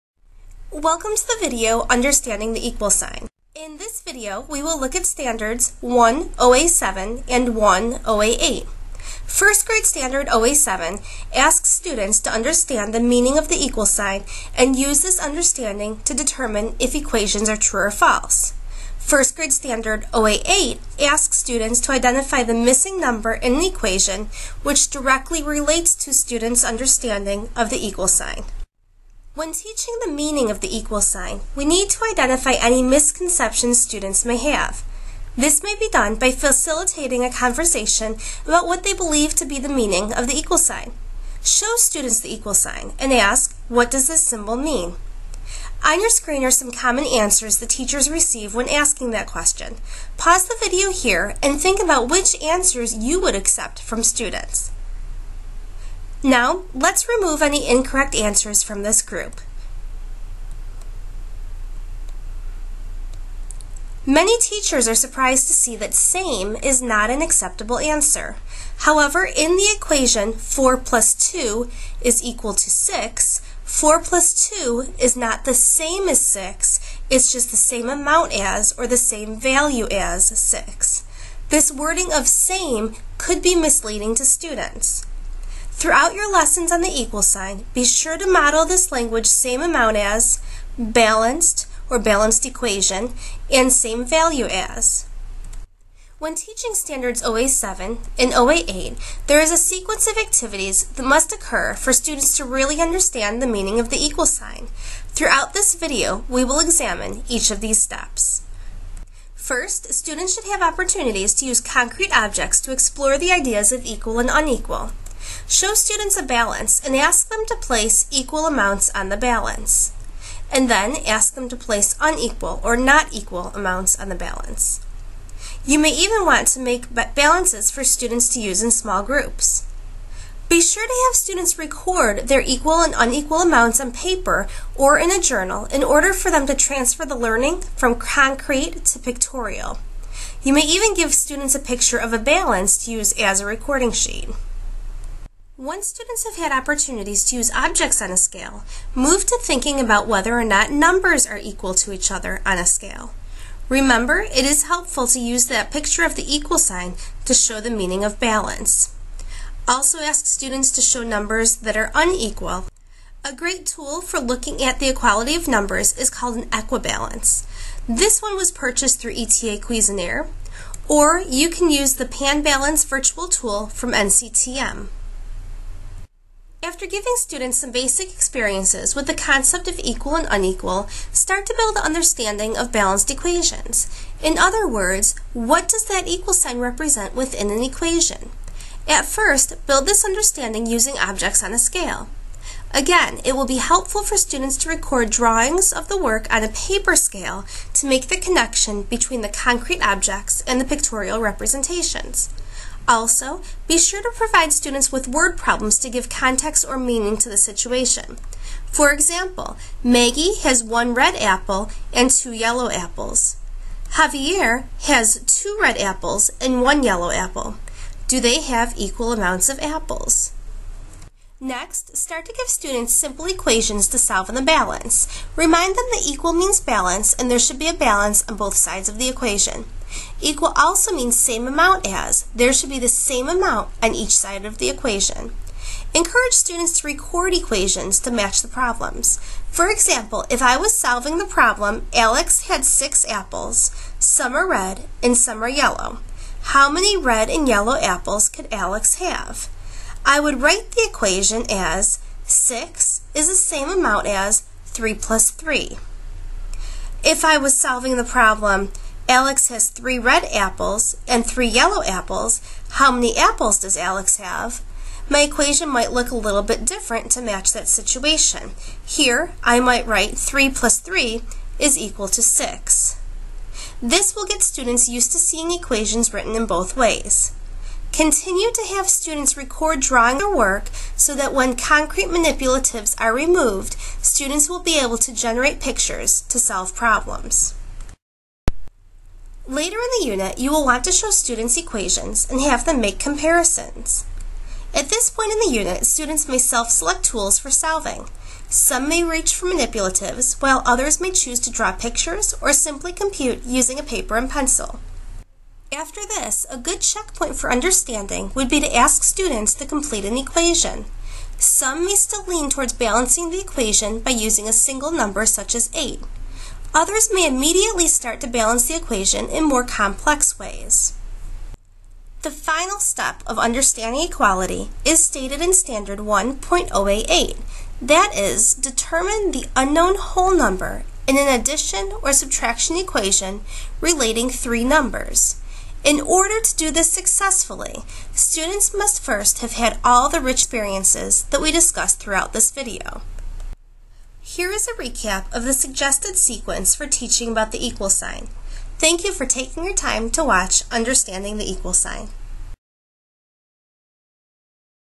Understanding the Equal Sign
This video describe a sequence of activities for introducing the equal sign to first graders.  The focus is on standards 1.OA.7 and 1.OA.8.